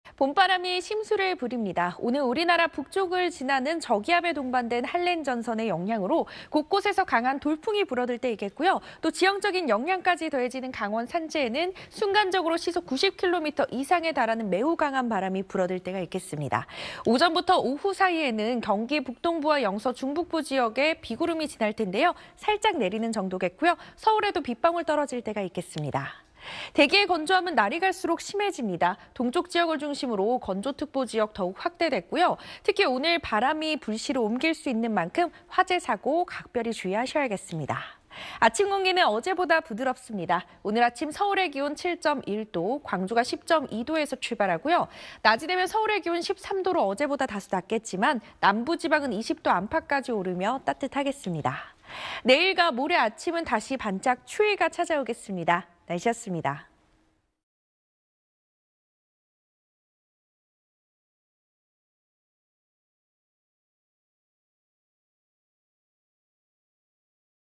[날씨] 순간 강한 돌풍 주의…내일 아침, 다시 반짝 추위 / SBS
봄바람이 심술을 부립니다.

오늘(15일) 우리나라 북쪽을 지나는 저기압에 동반된 한랭전선의 영향으로 곳곳에서 강한 돌풍이 불어들 때 있겠고, 또 지형적인 영향까지 더해지는 강원 산지에는 순간적으로 시속 90km 이상에 달하는 매우 강한 바람이 불어들 때가 있겠습니다.

오전부터 오후 사이에는 경기 북동부와 영서 중북부 지역에 비구름이 지날 텐데요 살짝 내리는 정도겠고, 서울에도 빗방울 떨어질 때가 있겠습니다.

대기의 건조함은 날이 갈수록 심해집니다.

동쪽 지역을 중심으로 건조특보 지역 더욱 확대됐고, 특히 오늘 바람이 불씨로 옮길 수 있는 만큼 화재 사고 각별히 주의하셔야겠습니다.

아침 공기는 어제보다 부드럽습니다.

오늘 아침 서울의 기온 7.1도, 광주가 10.2도에서 출발하고, 낮이 되면 서울의 기온 13도로 어제보다 다소 낮겠지만 남부지방은 20도 안팎까지 오르며 따뜻하겠습니다.

내일과 모레 아침은 다시 반짝 추위가 찾아오겠습니다.

(안수진 기상캐스터)